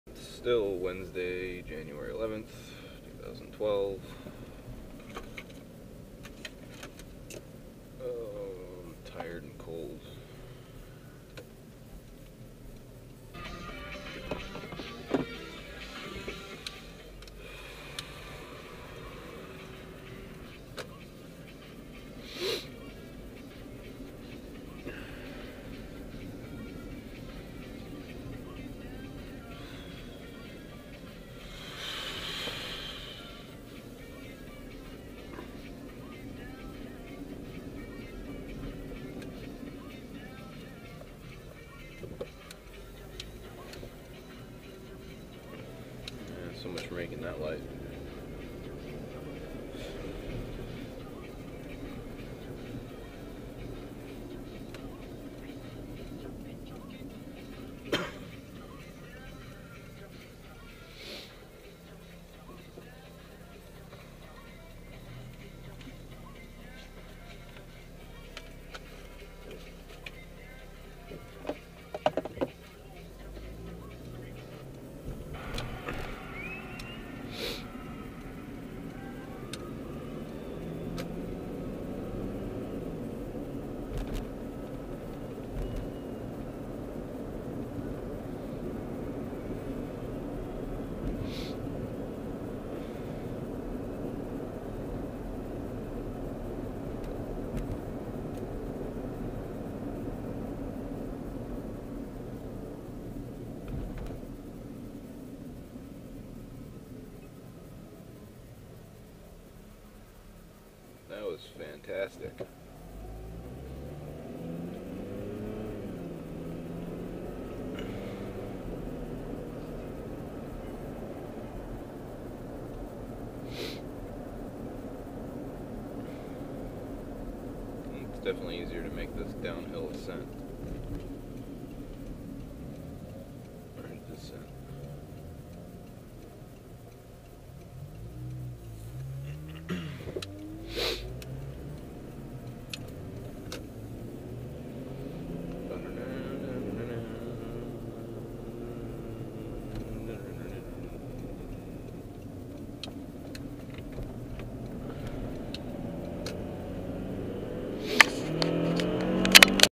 2012.01.11 Daily Driving: Saddleback, Food, Home [6/9]
Drove to Saddleback today, got food on the way home. Driving around now with MORE commentary! Sometimes even special Guests. Just an example video from my Contour HD camera to show off the video quality to friends. You might want to turn down your speakers, the microphone is lacking.